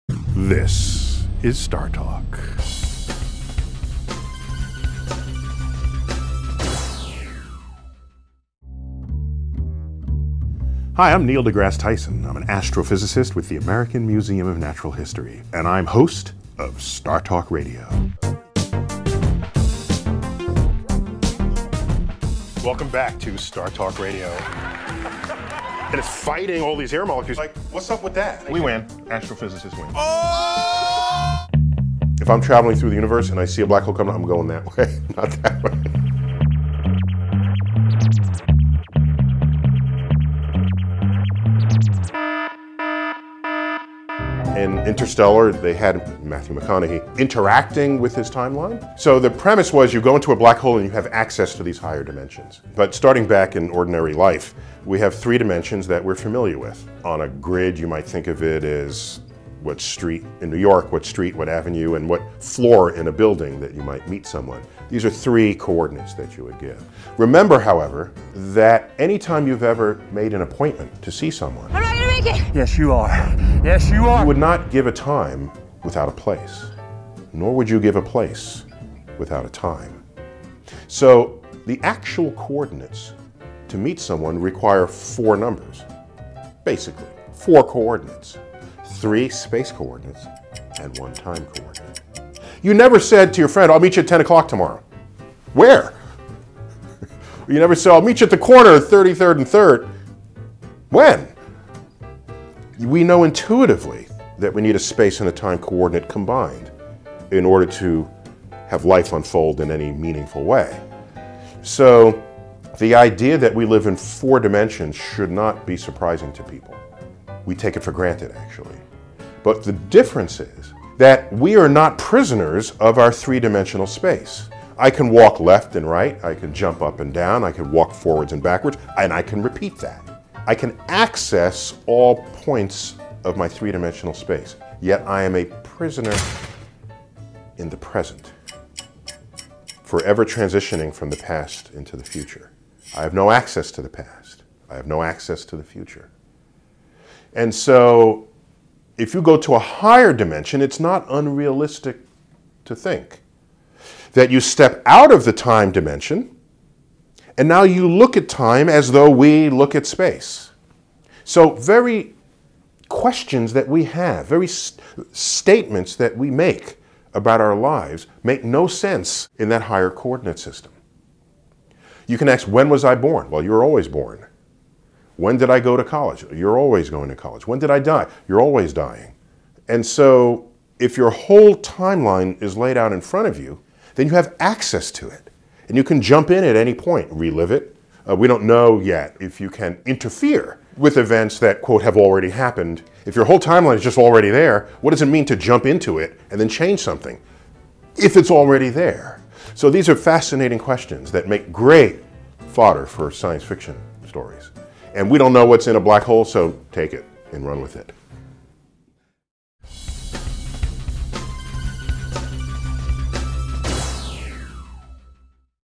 Neil deGrasse Tyson Discusses Time Travel in "Interstellar"
What would our own personal timelines look like if we could step out of the dimension of time and look at our lives the way we currently look at our location in physical space? Find out in this video from Business Insider, when StarTalk Radio host Neil deGrasse Tyson discusses time, time travel, higher dimensions and how they’re depicted in the movie “Interstellar.” (Spoiler alert.)